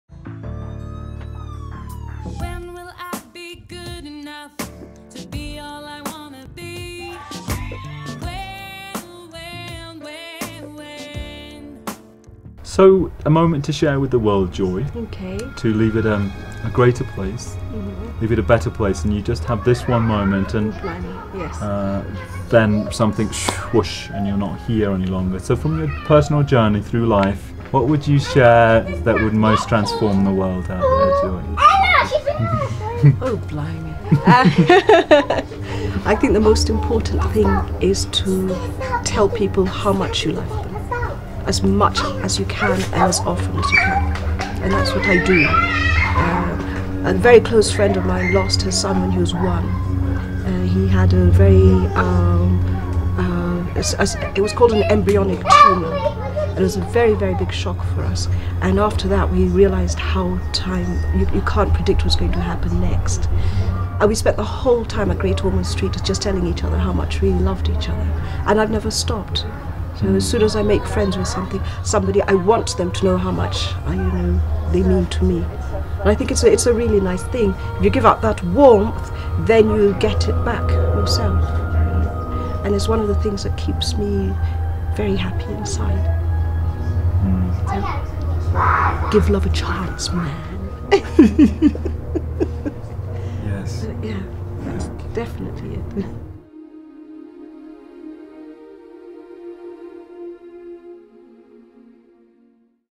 How Much I Love You Joy Kiano 2 of 2
Joy shares her moment – To Leave The World A Better Place, based on the story of a close friends son who died after one year of life.

Bear Dogz (David Roy Green) of WHEN whenworldwide.org interviews Joy Kiano in Nairobi, Kenya. Joy, daughter of Jane Kiano, and her husband were our amazing hosts at Murangi Homes where we stayed in a beautiful atmosphere in Karen, Nairobi.